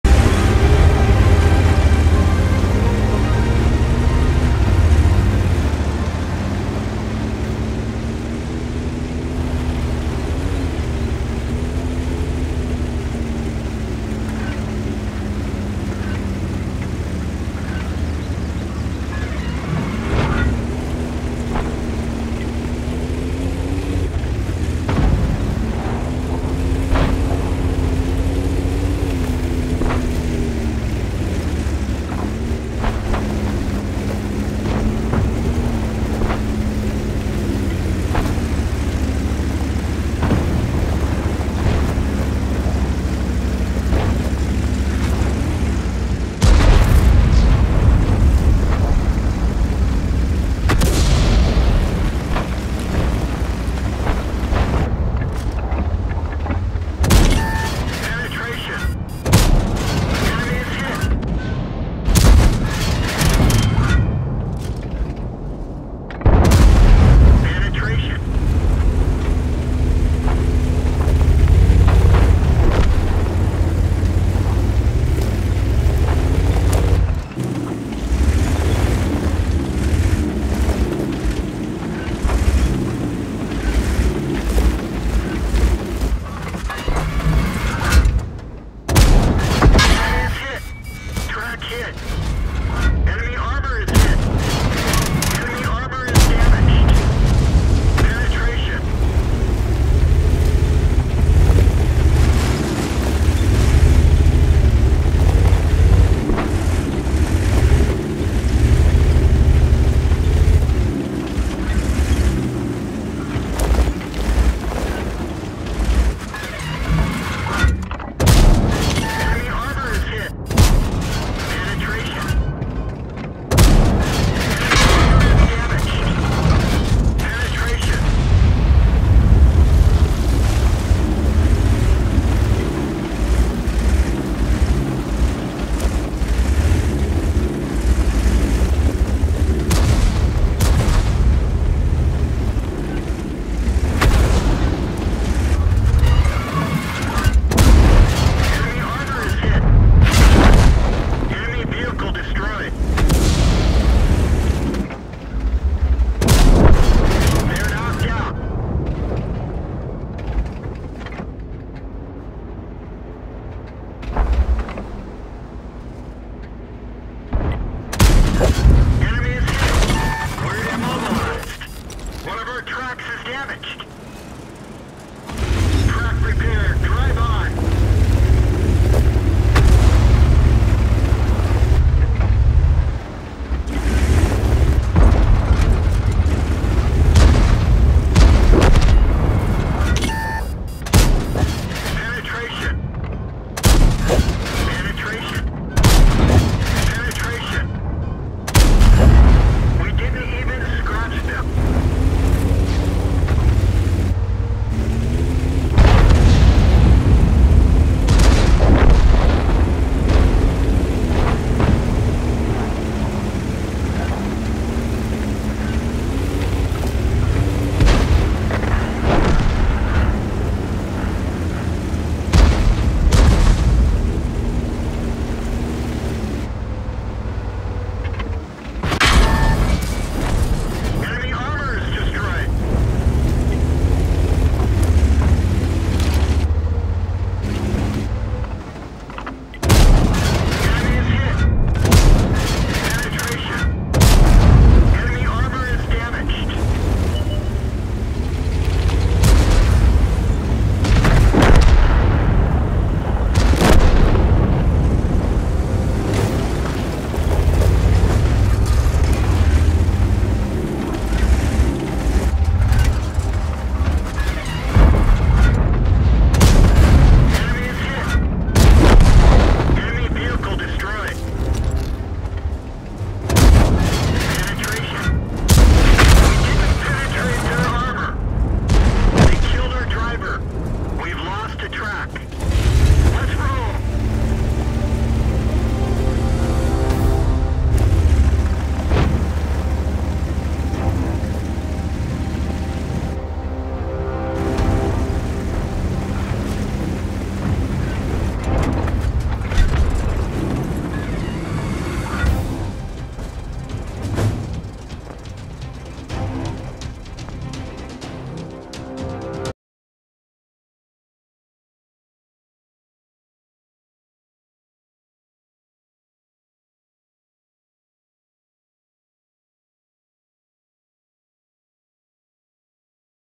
9к за 5 минут
Игрок: 3EJIbE [VNDLS]
9к за 5 минут

9к урона за 5 мин набил на хевике